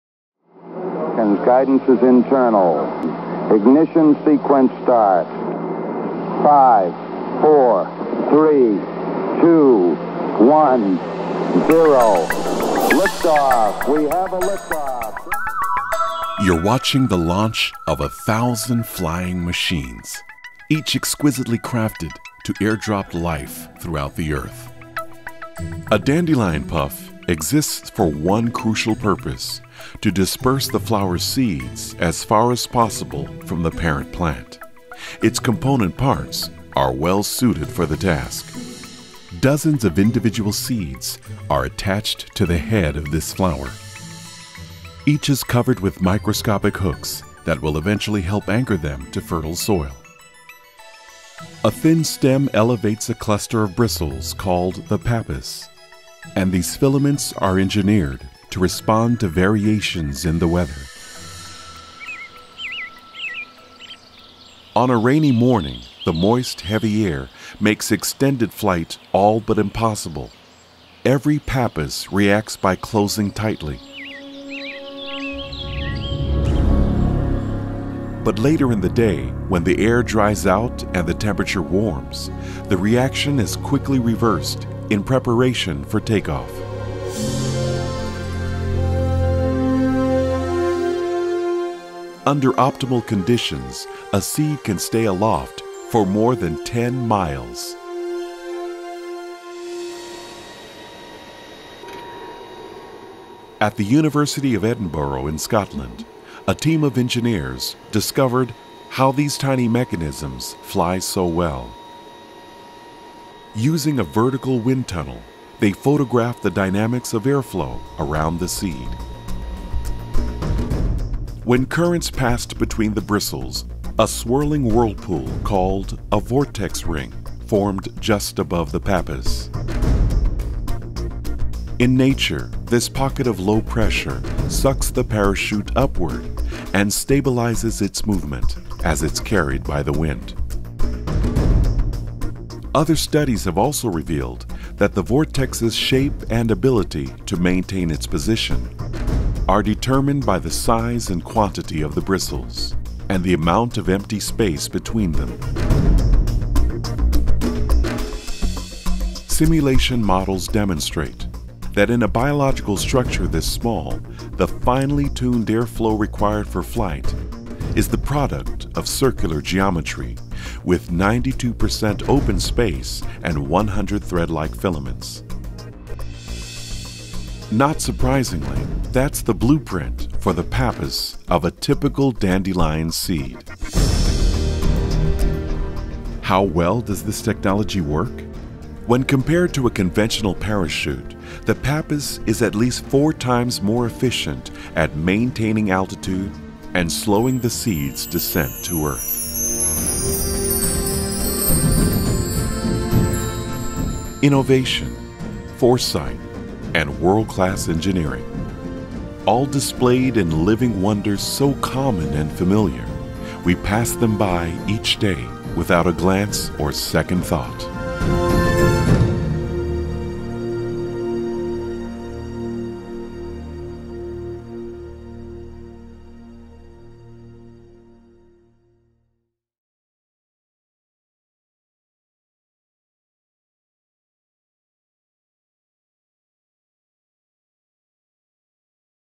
2-MINUTE WONDERS: Uplifting Story
Earth is filled with spectacular natural wonders and many of them are so common or small we pass by them each day without a glance or second thought. Vivid examples are the parachutes on the puff ball of a dandelion. Recent discoveries by an international team of scientists have revealed how these miniature aircraft can—under optimal conditions—remain aloft for miles. The dandelion seed’s aeronautical design is more efficient than any man-made parachute.